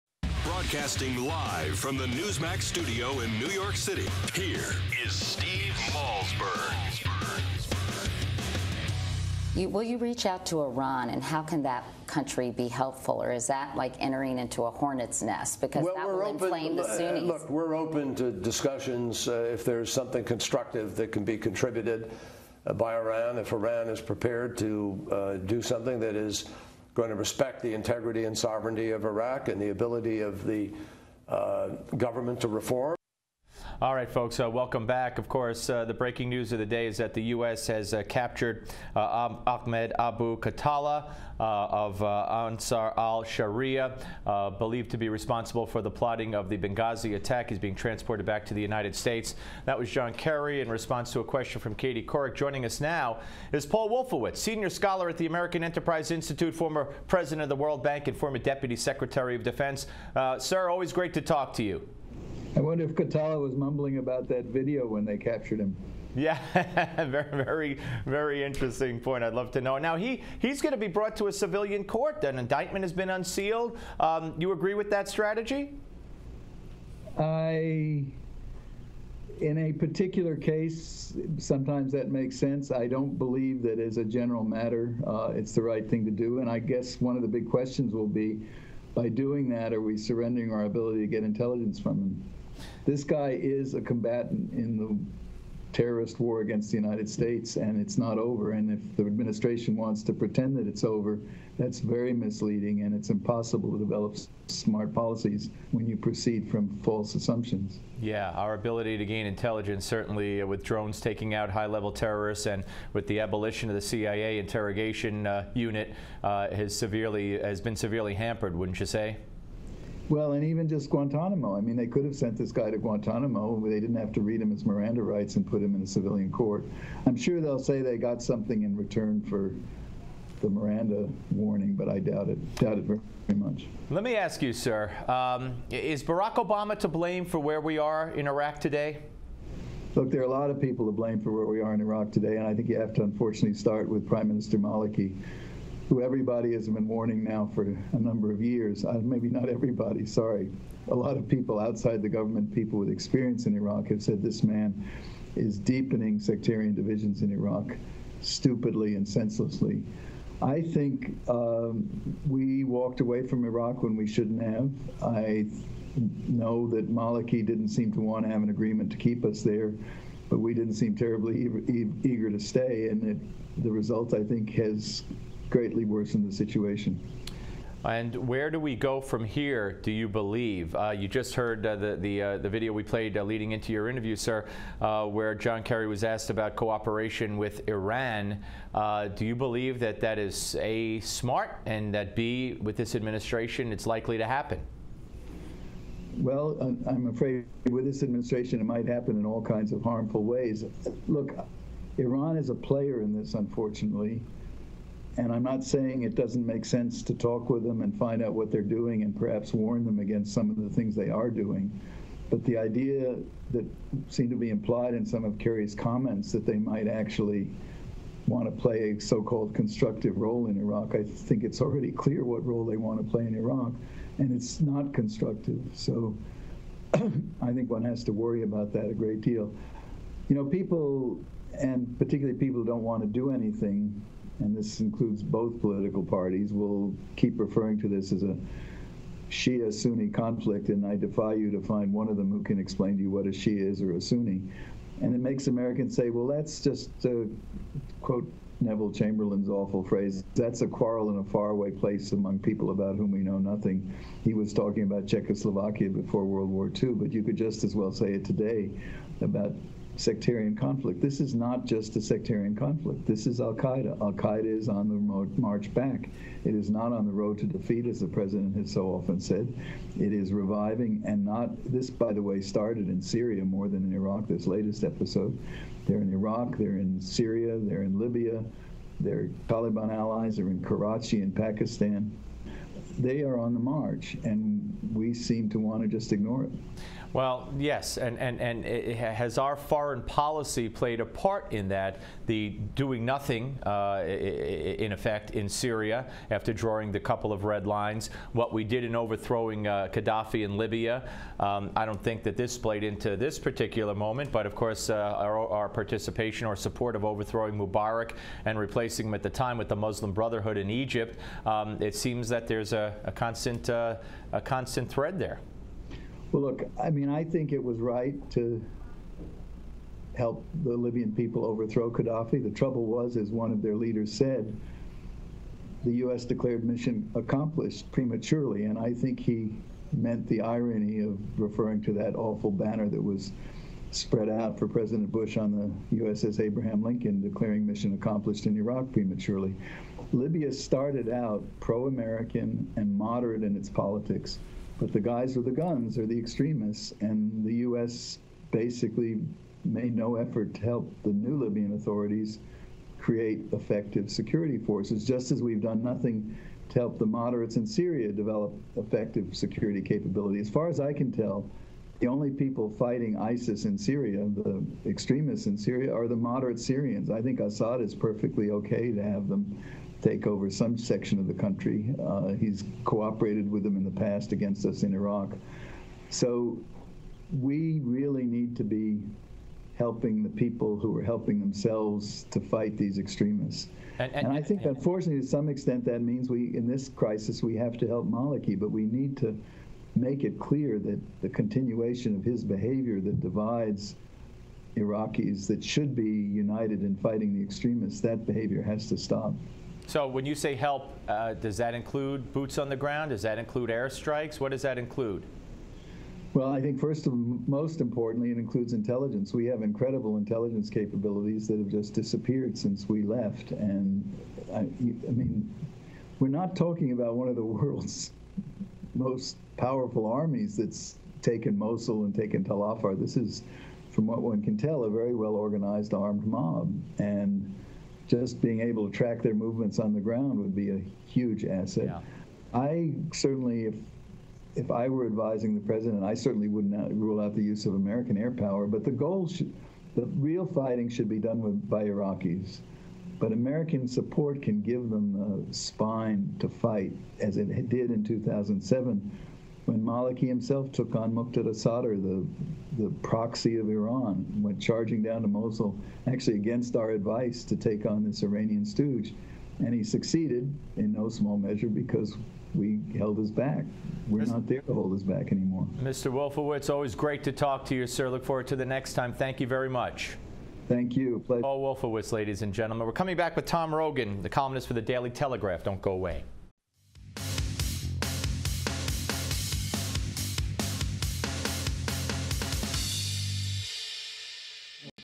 Paul Wolfowitz
senior scholar at the American Enterprise Institute, former president of The World Bank, and former deputy secretary of defense joins Steve to share his take on whom, if anyone, is to blame for the Iraq crisis